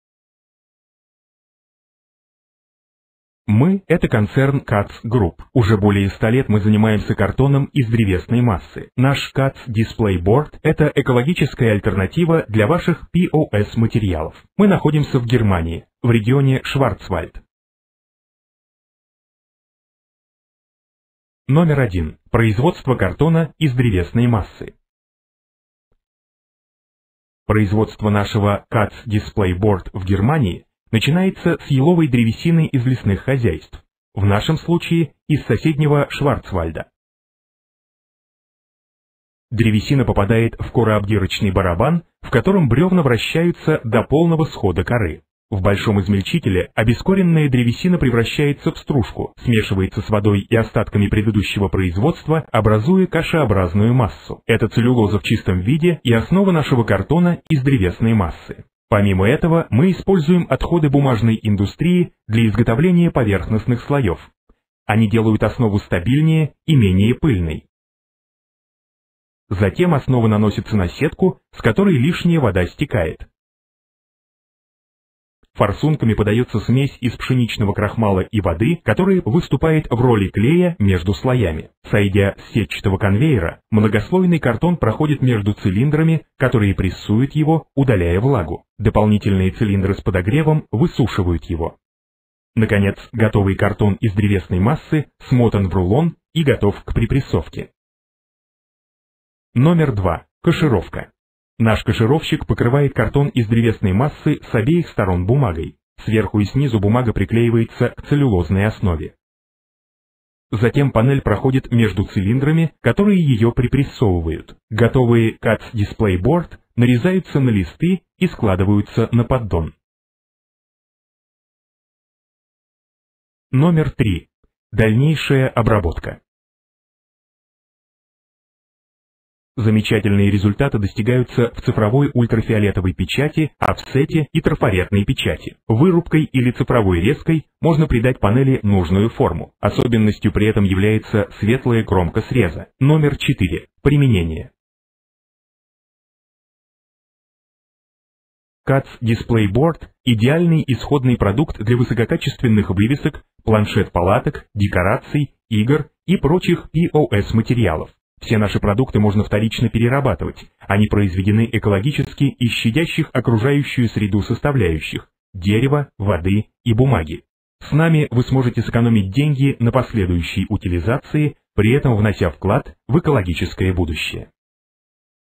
The KATZ Group -- KATZ DISPLAY BOARD -- production | выработка -- russian | российский
Manufactured from environmentally friendly KATZ wood pulp board, these two-side laminated display boards are the perfect print product for top-quality signage, tent cards and other POS display solutions.

KATZ DISPLAY BOARDS ARE THE IDEAL ECO-FRIENDLY CHOICE FOR
PEOPLE WHO CONSIDER THE FOLLOWING CRITERIA TO BE IMPORTANT:
• finding a good alternative to plastic
• choosing eco-friendly materials
• reducing disposal costs
• achieving optimum image quality and a clear, sharp edge
• finding a lightweight and stable solution
• a high quality environmentally friendly board product produced in Germany